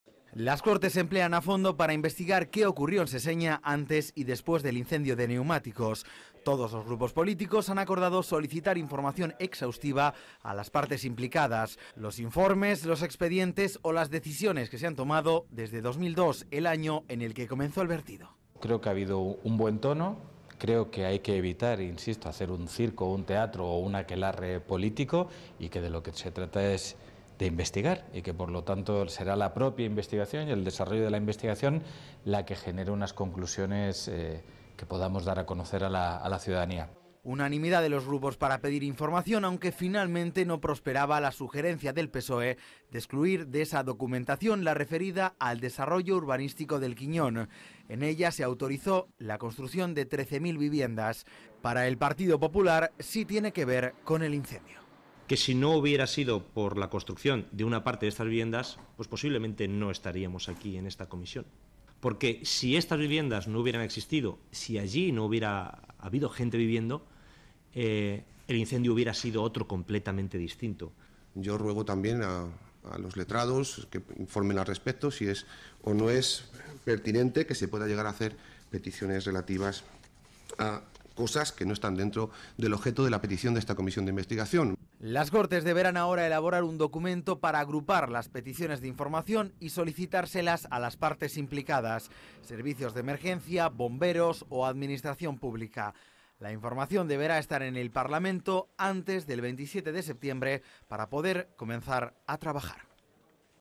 UNANIMIDAD EN LAS CORTES PARA SOLICITAR "TODA LA INFORMACIÓN" SOBRE EL VERTEDERO DE SESEÑA
Más de 100 documentos para conocer todo lo relativo al cementerio de neumáticos de Seseña. Con ellos trabajará la comisión de investigación que se ha creado en las Cortes de Castilla-La Mancha. Son informes, expedientes o decisiones que se han tomado desde que comenzó el vertido de ruedas en el año 2002. Los documentos deberán llegar al parlamento regional antes del 27 de septiembre.